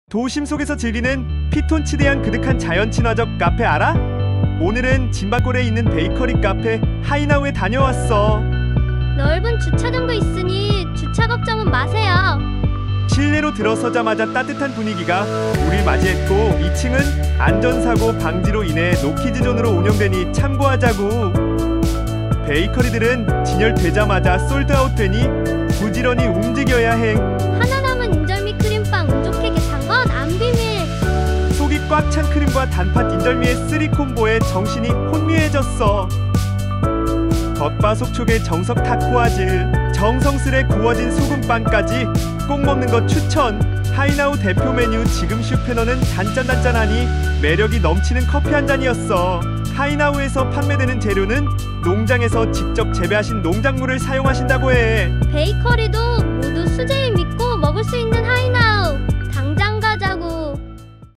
대구 진밭골 하이나우
진밭골에서 직접 키우고 기른 농작물과
과일들로 만드는 건강하고 맛있는 메뉴들!
하나하나 정성을 다해 좋은재료로 만들어
건강,맛 두마리 토끼 다 잡아버리네@@
도심속자연에서 즐기는 커피와 베이커리
안갈이유없지~ 이번주말은 하이나우로 고고!!!

📍 대구 수성구 진밭길 411-12 하이나우
🧭 10:00~22:00 (화~토요일)
🧭 10:00~19:00 (일요일)
❌ 매주 월요일 휴무 (공휴일의 월요일은 정상영업)